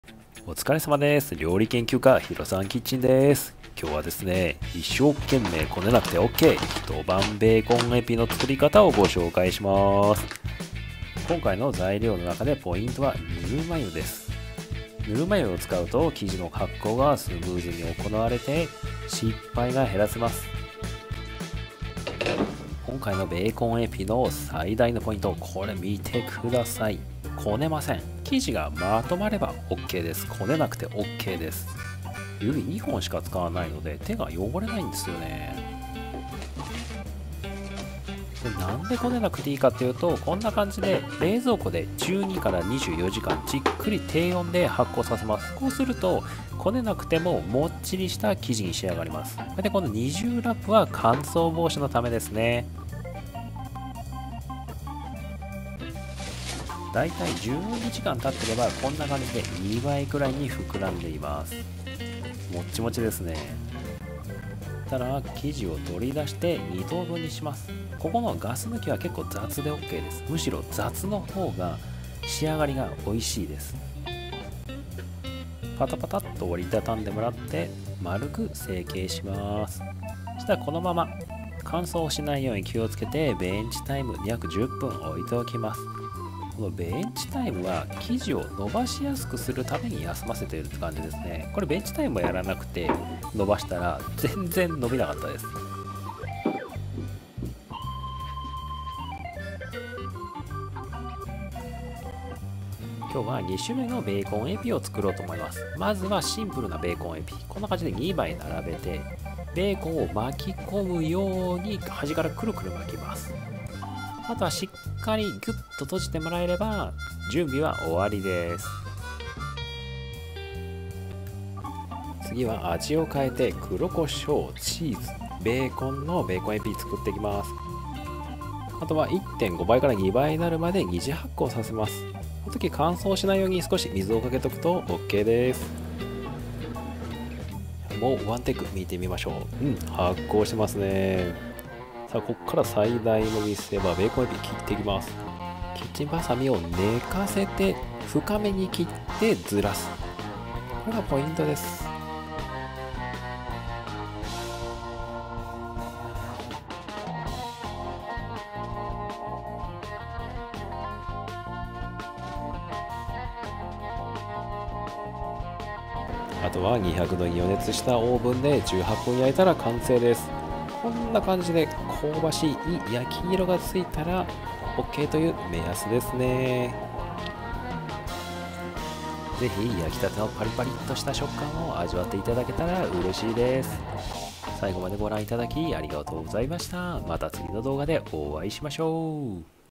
【こねないパン】もっちりベーコンエピの作り方
今日はベーコンエピの作り方を紹介します。こねなくてもこねるより美味しいパンの作り方す♪

僕のおすすめな方法「オーバーナイト発酵」方法です。少ないイーストで冷蔵庫を使ってじっくり発酵させます。これでイースト臭くなく小麦粉の旨味を引き出されます。

イーストの使用量は普通のパンを作るときの半分量になるので、節約にもなっちゃいます。

他のパン作りにも応用できる優れた方法なので、取り入れていただけたら嬉しいです♪

オープニング 0:00
材料の確認 0:09
作り方 0:15
パンを成型 1:04
ベーコンを巻く 1:41
ベーコンエピを焼く 2:48

材料
強力粉150g、薄力粉50g、砂糖12g、塩3g、ドライイースト2g、ぬるま湯145ml、ベーコン、チーズ、胡椒